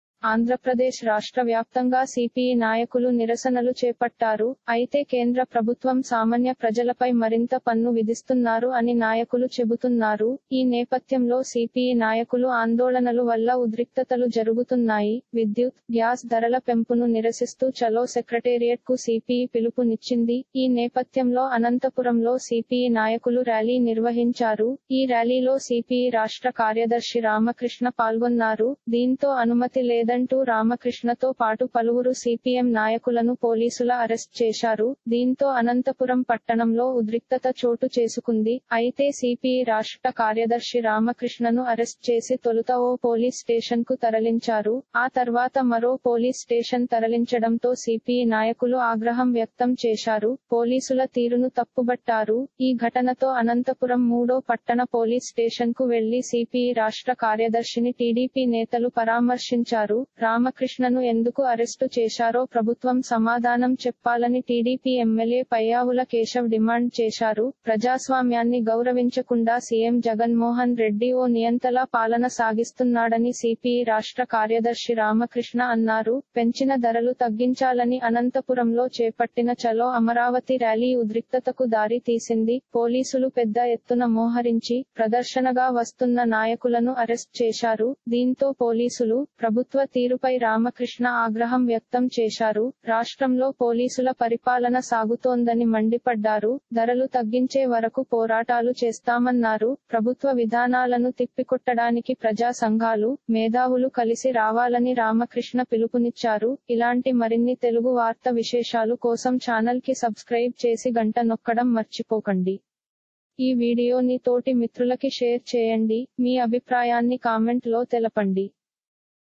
Will The Statewide Protest By CPI Leaders Pay Off - రాష్ట్రవ్యాప్తంగా సీపీఐ నాయకుల నిరసన ఫలిస్తుం
ఆంధ్రప్రదేశ్ రాష్ట్ర వ్యాప్తంగా సీపీఐ నాయకులు నిరసనలు చేపట్టారు. అయితే కేంద్ర ప్రభుత్వం సామన్య ప్రజలపై మరింత పన్ను విధిస్తున్నారు అని నాయకులు చెబుతున్నారు. ఈ నేపథ్యంలో సీపీఐ నాయకులు ఆందోళనలు వల్ల ఉద్రిక్తతలు జరుగుతున్నాయి.

విద్యుత్, గ్యాస్ ధరల పెంపును నిరసిస్తూ చలో సెక్రటేరియట్ కు సీపీఐ పిలుపు నిచ్చింది. ఈ నేపథ్యంలో అనంతపురంలో సీపీఐ నాయకులు ర్యాలీ నిర్వహించారు. ఈ ర్యాలీలో సీపీఐ రాష్ట్ర కార్యదర్శి రామకృష్ణ పాల్గొన్నారు. దీంతో అనుమతి లేదంటూ రామకృష్ణతో పాటు పలువురు సీపీఎం నాయకులను పోలీసుల అరెస్ట్ చేశారు. దీంతో అనంతపురం పట్టణంలో ఉద్రిక్తత చోటు చేసుకుంది.

అయితే సీపీఐ రాష్ర్ట కార్యదర్శి రామకృష్ణను అరెస్ట్ చేసి తొలుత ఓ పోలీస్ స్టేషన్‎కు తరలించారు. ఆ తర్వాత మరో పోలీస్ స్టేషన్ తరలించడంతో సీపీఐ నాయకులు ఆగ్రహం వ్యక్తం చేశారు. పోలీసుల తీరును తప్పుబట్టారు. ఈ ఘటనతో అనంతపురం 3వ పట్టణ పోలీస్ స్టేషన్‎కు వెళ్లి  సీపీఐ రాష్ట్ర కార్యదర్శిని టీడీపీ నేతలు పరామర్శించారు. రామకృష్ణను ఎందుకు అరెస్టు చేశారో ప్రభుత్వం సమాధానం చెప్పాలని టీడీపీ ఎమ్మెల్యే పయ్యావుల కేశవ్ డిమాండ్ చేశారు.

ప్రజాస్వామ్యాన్ని గౌరవించకుండా సీఎం జగన్మోహన్ రెడ్డి ఓ నియంతలా పాలన సాగిస్తున్నాడని సీపీఐ రాష్ట్ర కార్యదర్శి రామకృష్ణ అన్నారు. పెంచిన ధరలు తగ్గించాలని అనంతపురంలో చేపట్టిన చలో అమరావతి ర్యాలీ ఉద్రిక్తతకు దారి తీసింది. పోలీసులు పెద్ద ఎత్తున మోహరించి, ప్రదర్శనగా వస్తున్న నాయకులను అరెస్ట్ చేశారు.

దీంతో పోలీసులు, ప్రభుత్వ తీరుపై రామకృష్ణ ఆగ్రహం వ్యక్తం చేశారు. రాష్ట్రంలో పోలీసుల పరిపాలన సాగుతోందని మండిపడ్డారు. ధరలు తగ్గించే వరకు పోరాటాలు చేస్తామన్నారు. ప్రభుత్వ విధానాలను తిప్పికొట్టడానికి ప్రజా సంఘాలు, మేధావులు కలిసి రావాలని రామకృష్ణ పిలుపునిచ్చారు. - Will The Statewide Protest By CPI Leaders Pay Off $telugu-title:రాష్ట్రవ్యాప్తంగా సీపీఐ నాయకుల నిరసన ఫలిస్తుందా?$ *pid:2009036* - Telugu News TeluguNews TeluguBreaking Telugu TeluguStop | Telugu news